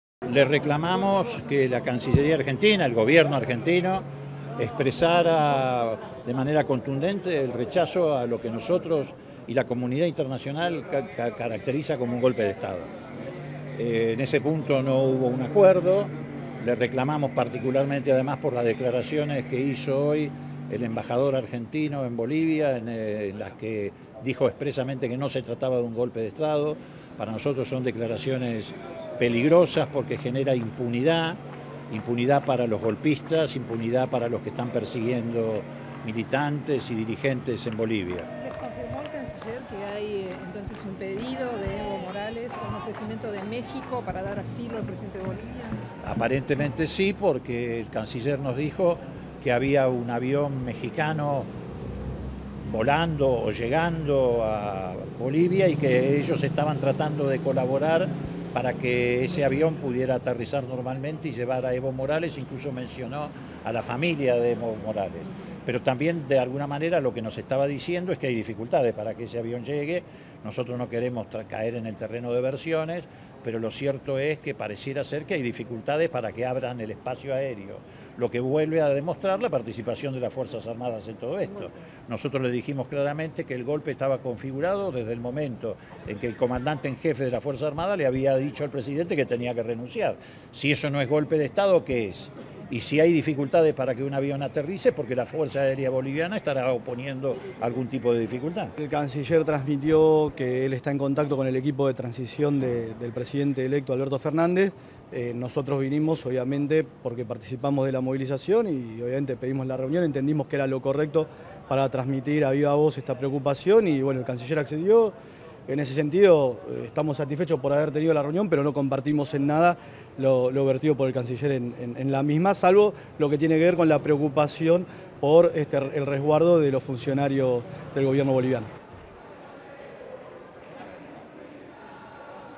Diputados K repudiaron la posición argentina sobre la crisis en Bolivia
Diputados del Frente de Todos se reunieron con Faurie en Cancillería y repudiaron la posición argentina sobre Bolivia.
Un grupo de diputados nacionales del Frente para La Victoria, encabezado por Leopoldo Moreau y Andrés "Cuervo" Larroque se reunió con el canciller Jorge Faurie.
Moreau: "Reclamamos que el Gobierno argentino expresara de manera contundente lo que la comunidad caracteriza como un golpe de Estado. No hubo un acuerdo... Dijimos que el golpe estaba configurado desde el momento en el que el comandante en jefe de las las Fuerzas Armadas le había dicho al presidente (Morales) que tenía que renunciar. Si eso no es golpe de Estado, ¿qué es?".
Larroque destacó que los diputados del FPV valoraron la reunión pero dijo que "no hubo acuerdo" sobre la caracterización de los hechos y las soluciones propuestas y que Faurie les dijo que está "en contacto con el equipo de transición del presidente electo Alberto Fernández".